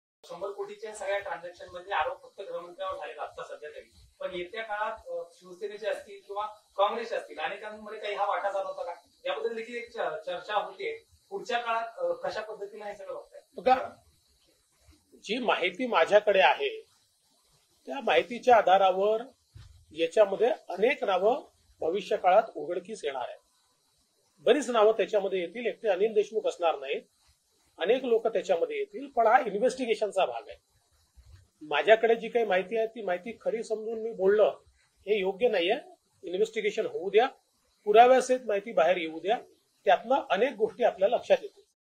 एकटे अनिल देशमुख नाही तर अनेक नावे यात उघडकीस येणार...
(पत्रकारांशी संवाद । मुंबई । दि. 5 एप्रिल 2021)